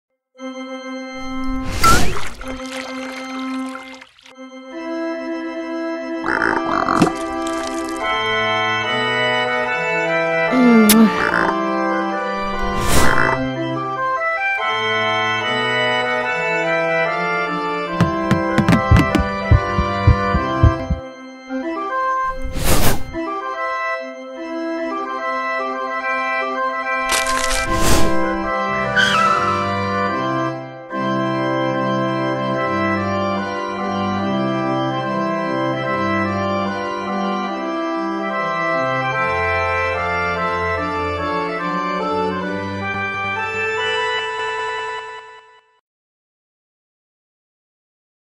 Invitation de mariage Numérique et personnalisé Crapaud Bleu
Modèle de faire-part de mariage 😍 numérique personnalisé Oui, je le veux !. Annoncez la date de votre mariagelink à votre famille et vos amis par Whatsapp.